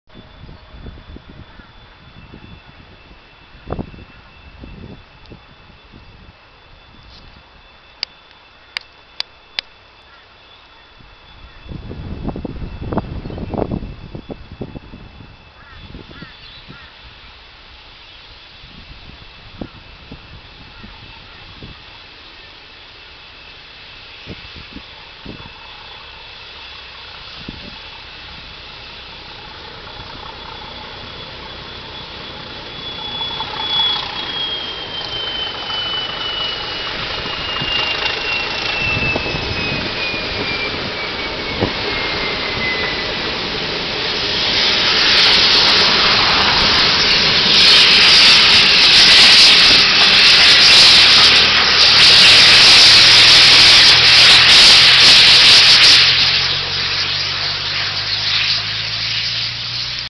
Special Steam train to Springfield returning to Darra
No turnback facilities at Springfield Central for locomotives so train was returned courtesy of DEL 1620.